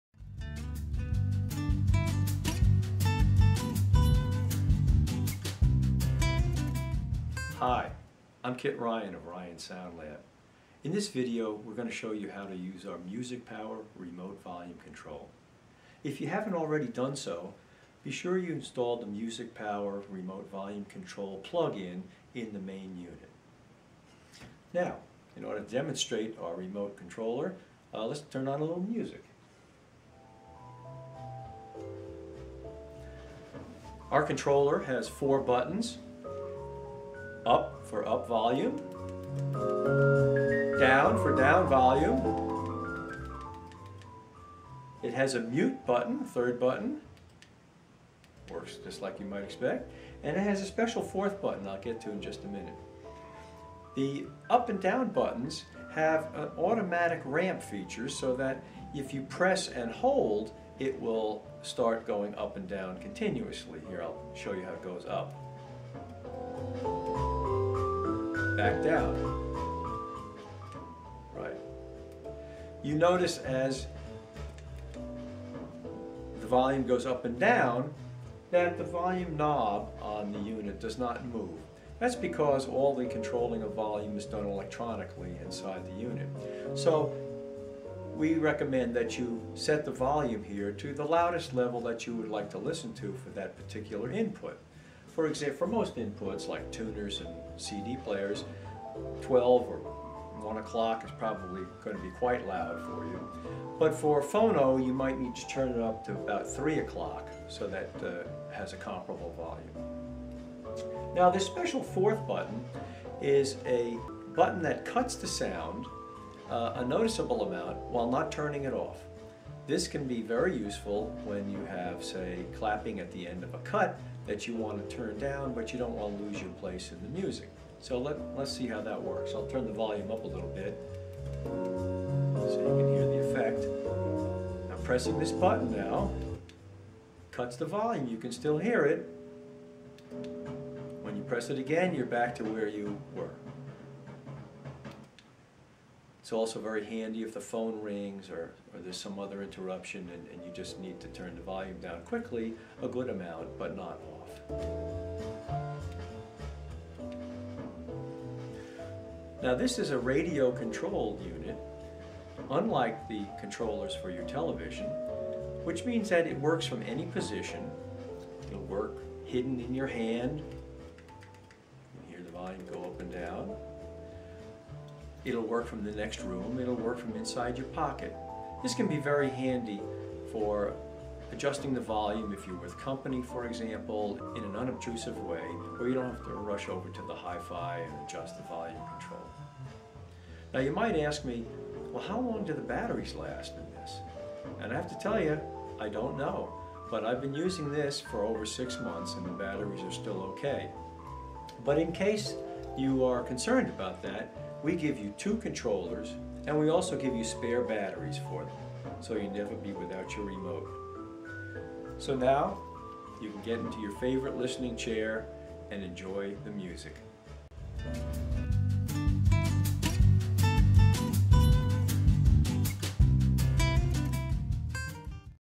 RemoteControlUserGuideVideo
User Guide for the RSL MusicPower remote volume control for classic Naim preamps.